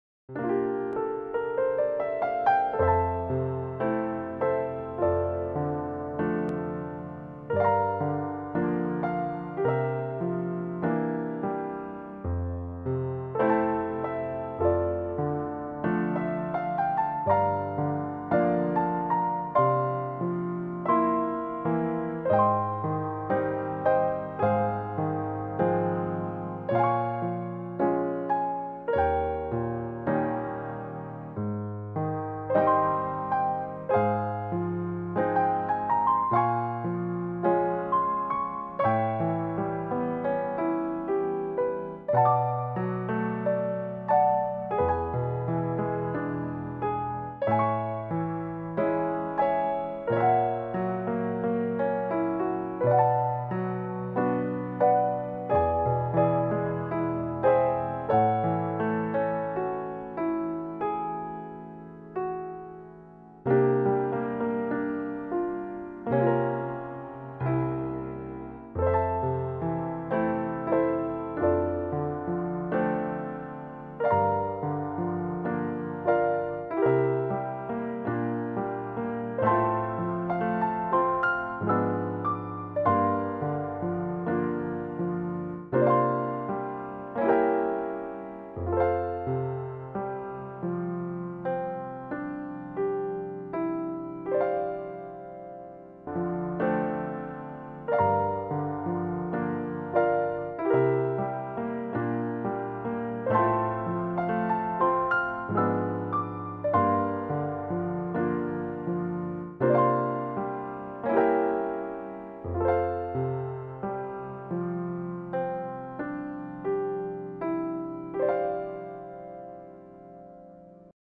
Morning Relaxing Music by Evergreen Relaxing Music channel designed to bright your mind and mornings
Morning Relaxing Music by Evergreen Relaxing Music channel specially designed to bright your mind and mornings. This video will help you to relax and make your mind stress free and to stay happy all day long.